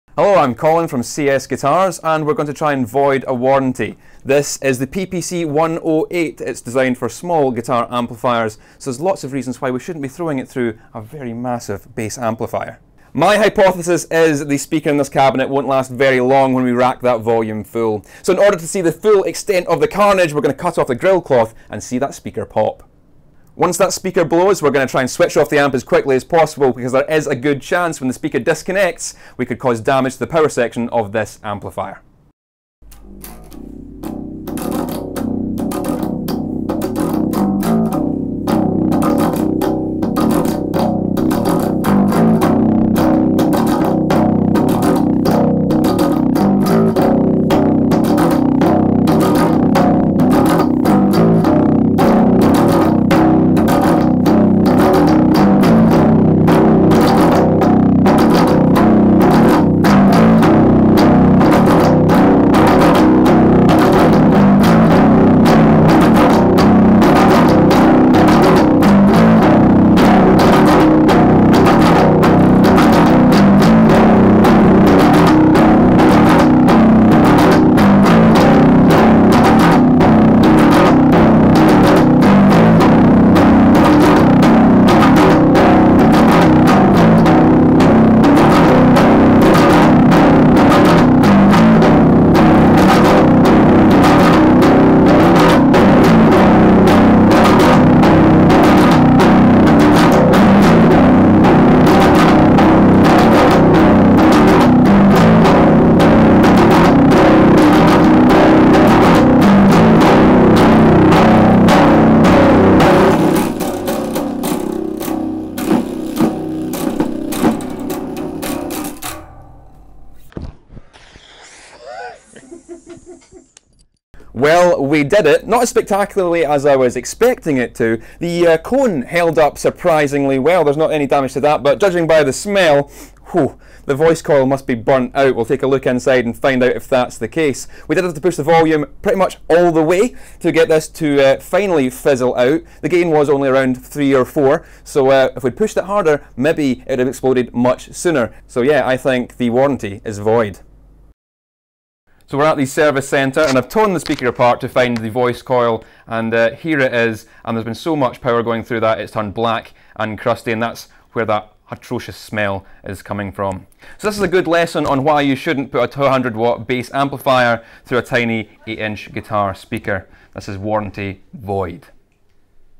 How to void your warranty... with POWER (S2 - E4)
What happens if you plug our most powerful amplifier into our smallest cabinet we hear you not say? Will it smoke? Will it blow up? Will it void your warranty?

We gave Colin from CS Guitars the task of plugging an AD200 into a PPC108 and play and this was the result.

Orange is answering all the questions you never asked in this series of How to void your warranty.